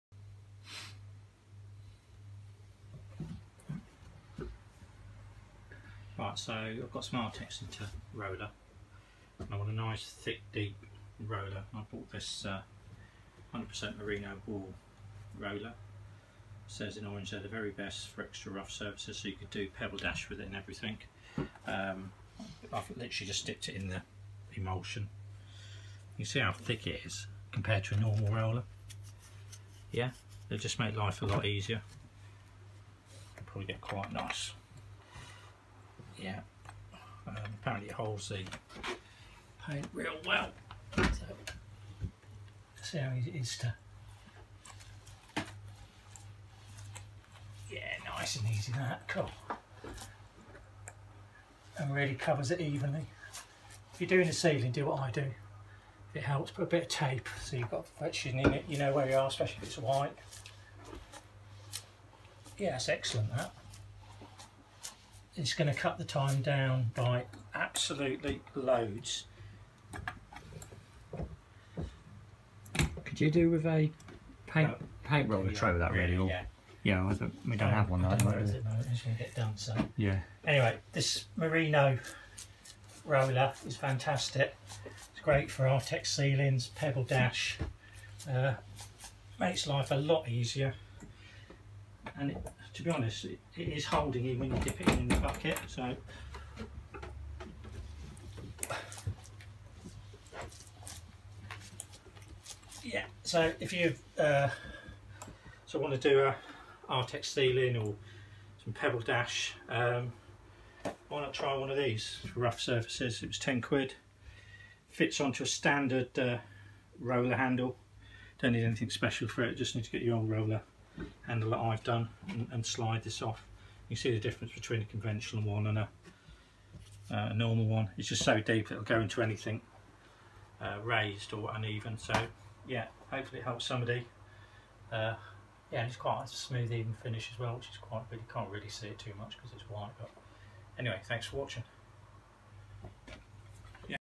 Axus Décor Long Merino Sheepskin Roller With Sleeve Pile Review
This roller offers the ability to cover uneven surfaces well without causing any damage due to the super-soft sheepskin it's made from. It fits a standard roller and can be washed, here is our experience with this product.